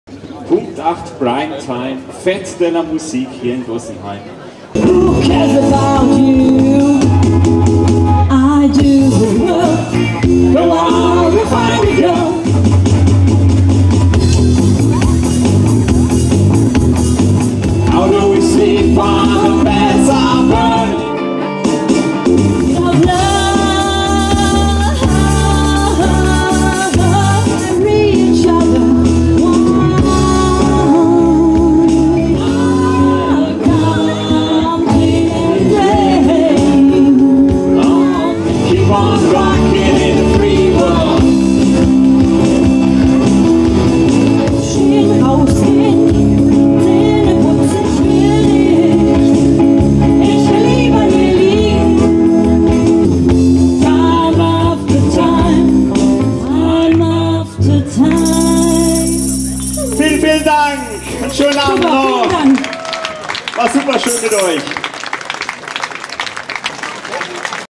Who Cares @ Fete de la musique 2022, Dossenheim
Auftritt auf der Fête de la musique in Dossenheim am 21. Juni 2022 .
Kleiner Zusammenschnitt

Who Cares sind:
Gabi: Vocals, Acoustic Guitar
Karsten: Vocals, Keyboard, Percussion
Markus: Electric Guitar
Thomas: Electric Bass
Tom@TheToms: Drums, Vocals